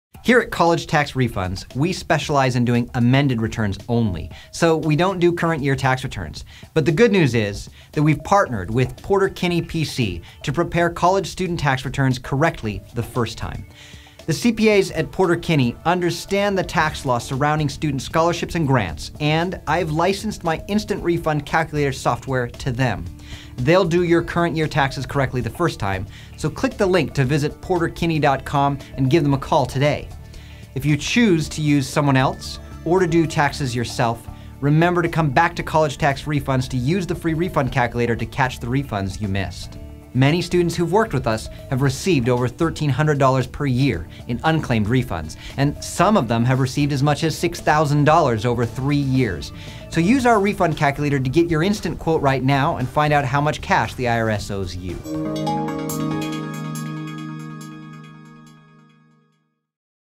Can you do my tax return this year?
We specialize in doing amended returns only. But we've partnered with the CPA's at Porter Kinney PC and given them exclusive access to our powerful refund calculator.